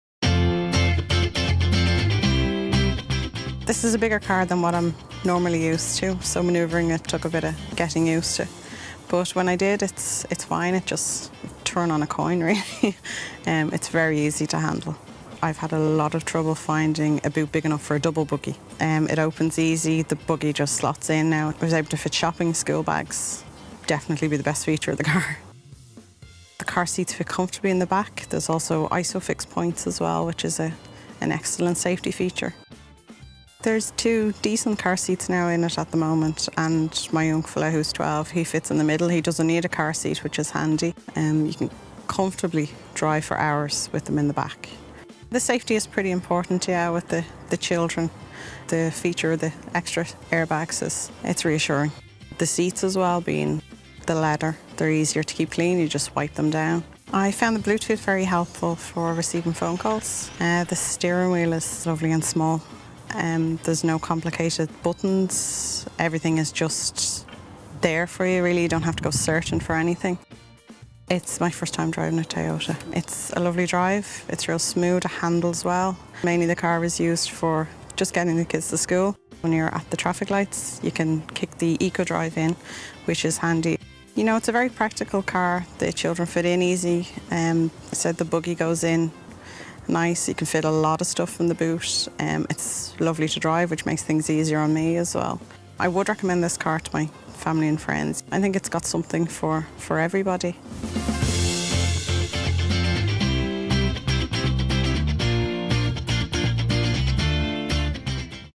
eumom member Jessica reviews the Toyota RAV4 after a week long test drive.
eumom member Jessica tells us how she and her children got on driving around in the Toyota RAV4 for a week.

"It's a very practical car ... You can fit a lot in and it's lovely to drive ... something for everybody. "

The RAV4 is available with Toyota Flex Car Finance from €299 a month*